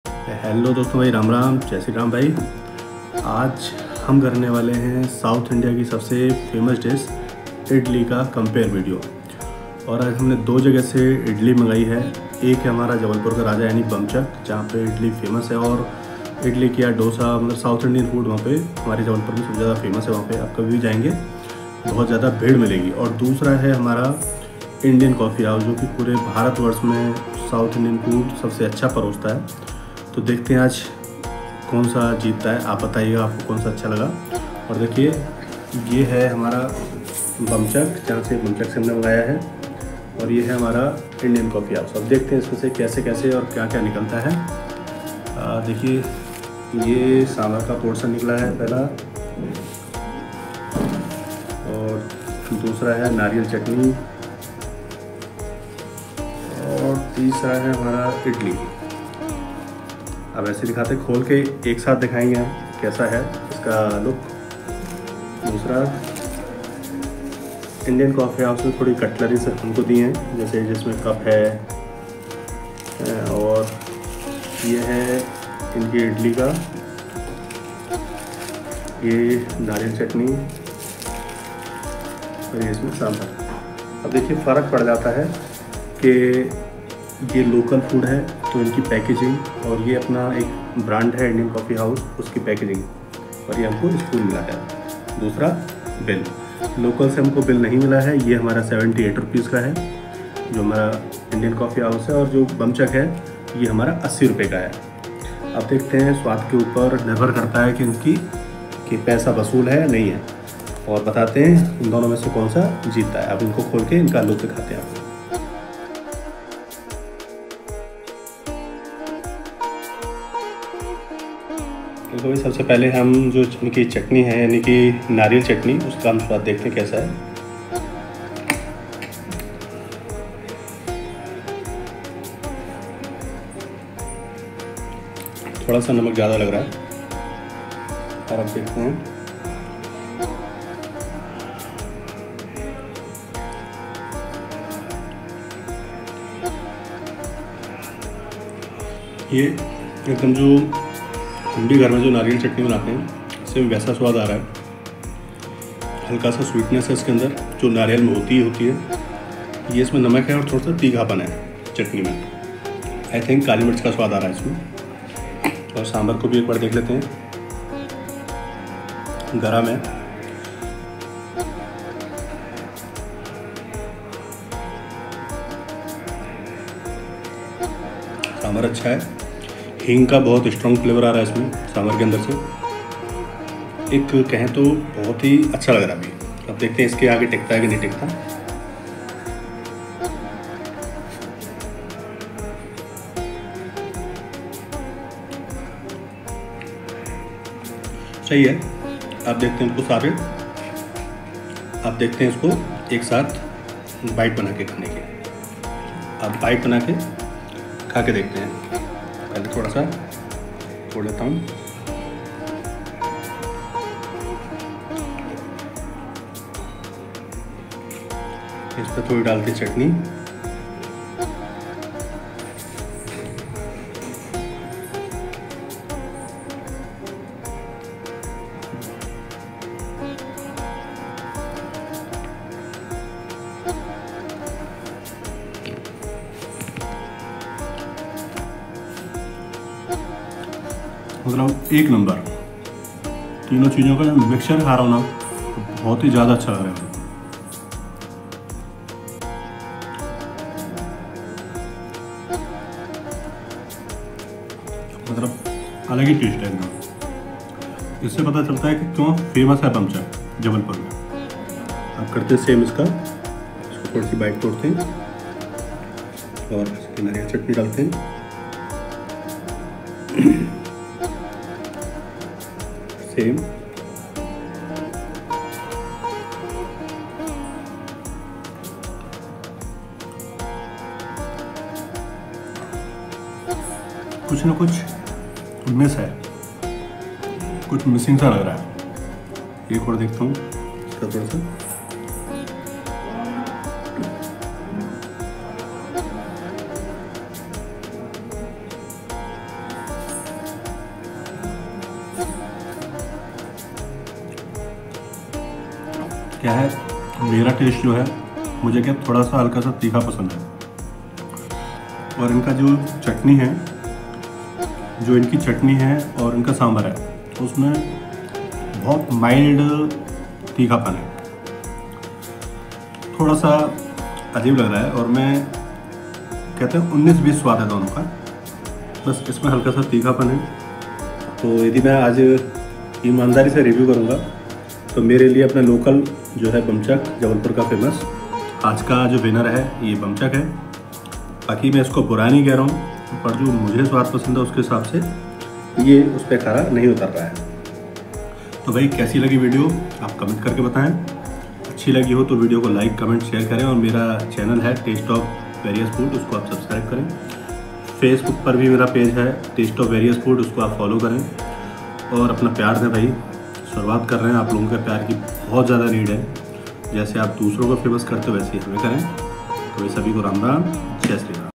Best Idli in jabalpur, Local 🆚 Indian Coffee House! who will win
once again a comparison video, this time idli compares video, Local 🆚 Indian Coffee House